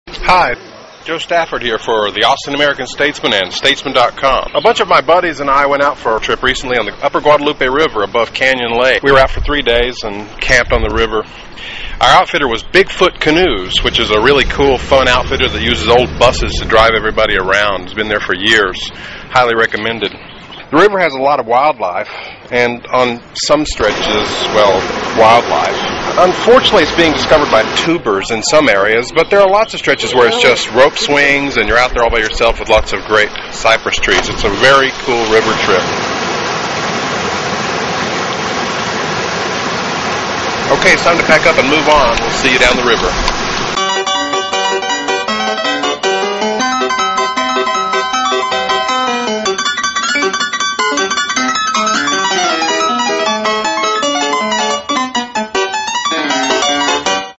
Guadalupe River canoe trip
American-Statesman staffer Joe Stafford and friends spend three days on the upper Guadalupe River. A one-minute video.